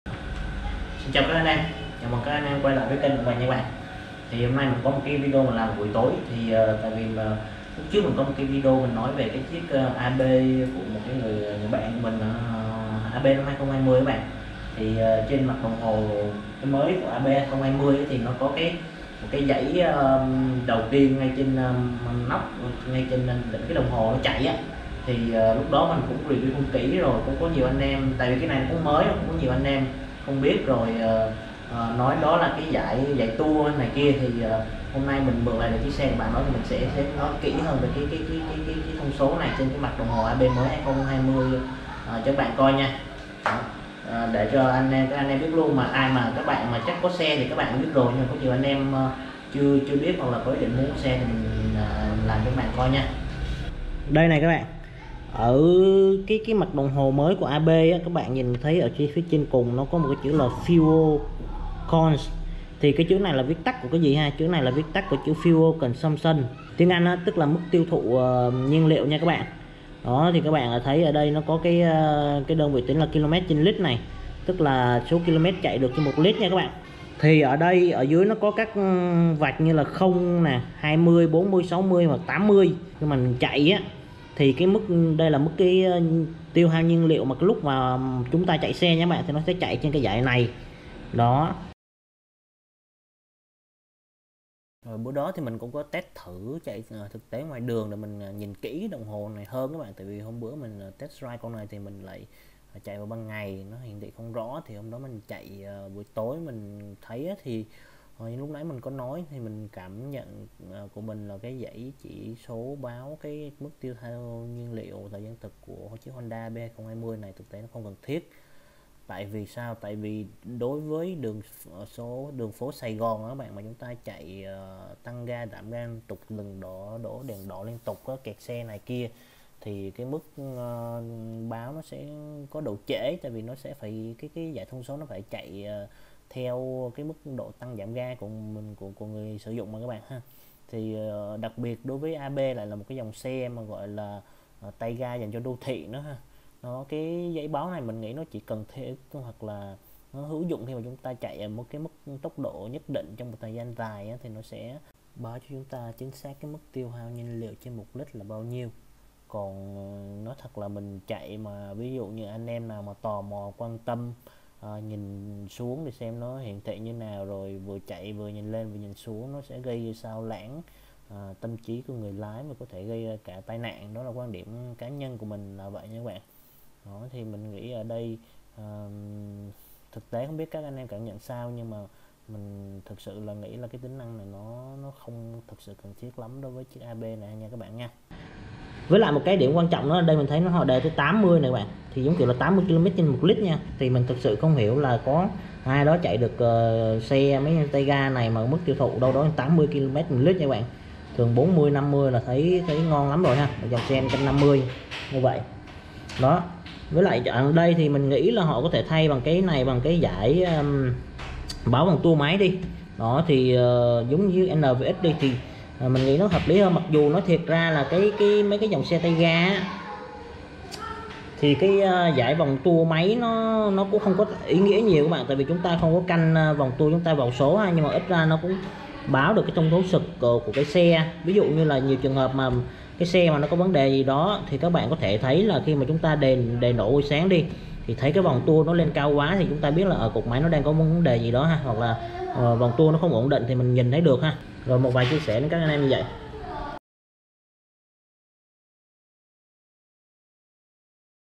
Đồng hồ Airblade 2020 - dải báo Fuel Cons là gì và có thật sự cần thiết?
Anh em có thể chia sẻ thêm về cảm nhận cùa mình về dải đồng hồ này của con AB 2020 nhé!